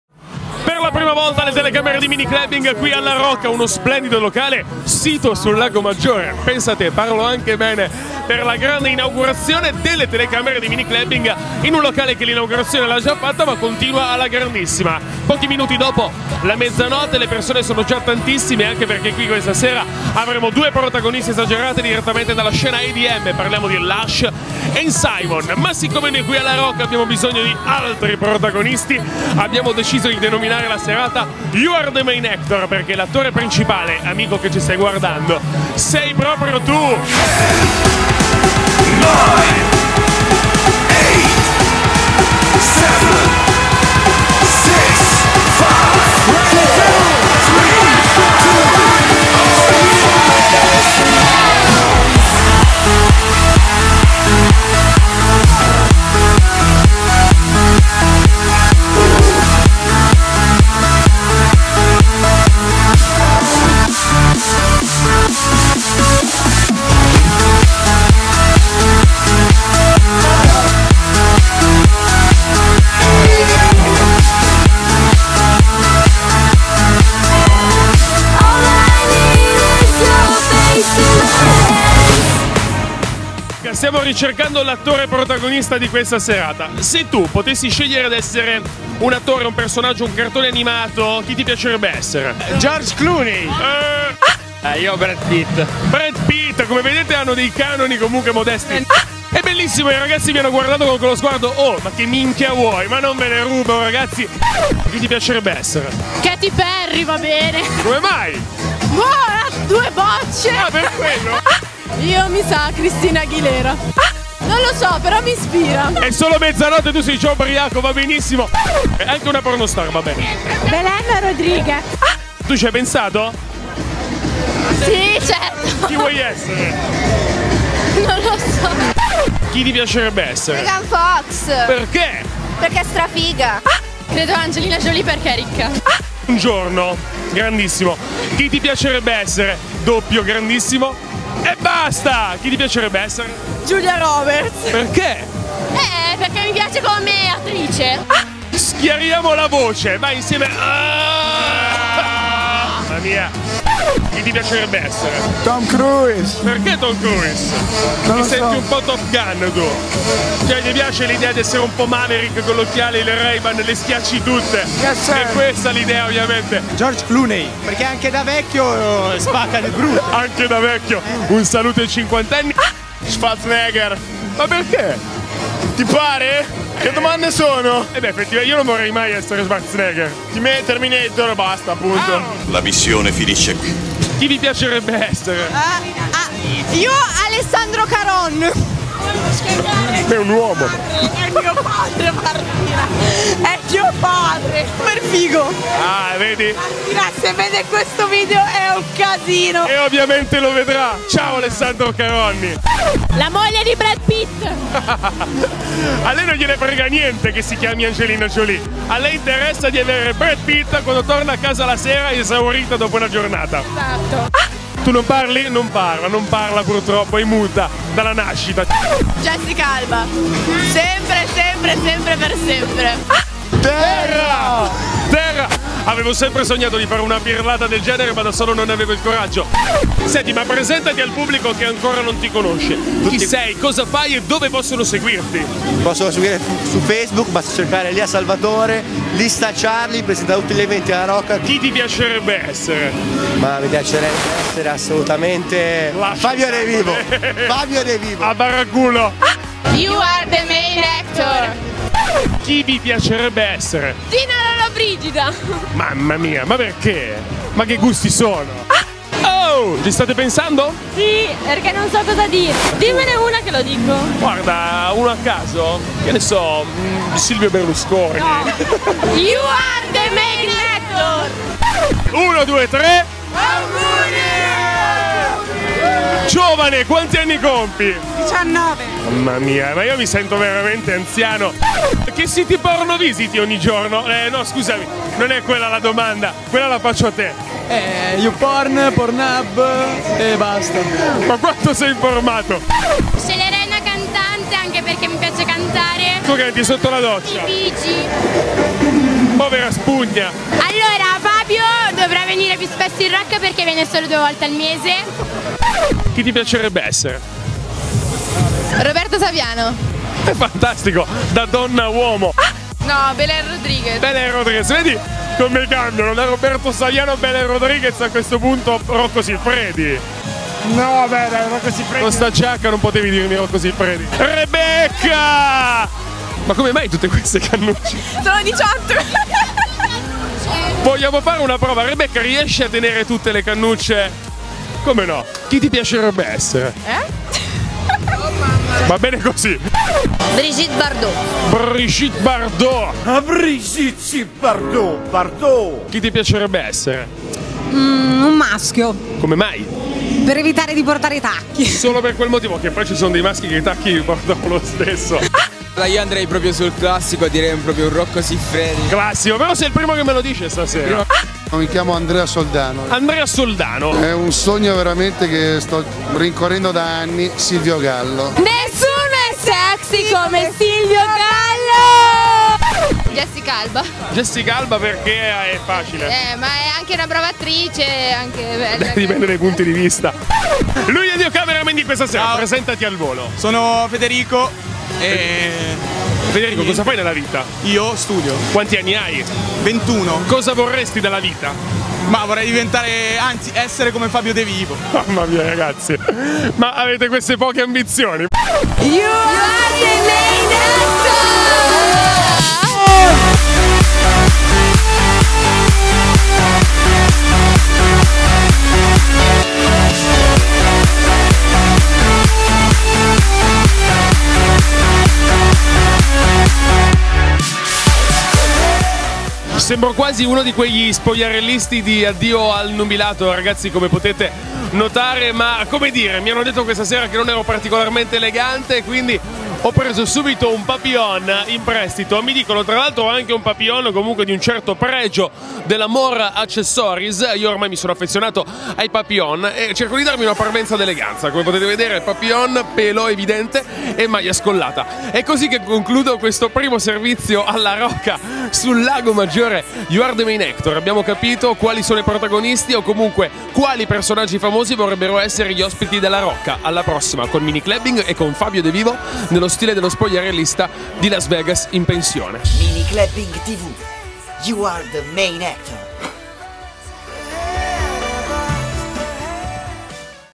Ma chi ti credi di essere?!? By LA ROCCA
Prima puntata di MINICLUBBING, condotta a LA ROCCA di Arona: splendido club della provincia di Novara, a ridosso del Lago Maggiore.

In serata, tante interviste surreali condotte da FABIO DE VIVO a ritmo di "CHI TI PIACEREBBE ESSERE" e il Sound del duo Italiano, ma Super Internazionale "LUSH & SIMON".

Guarda, commenta e condividi questo video!!!

Servizio di FABIO DE VIVO
Location: LA ROCCA
Riprese di FEDERICO STILO
Prodotto da MINICLUBBING TV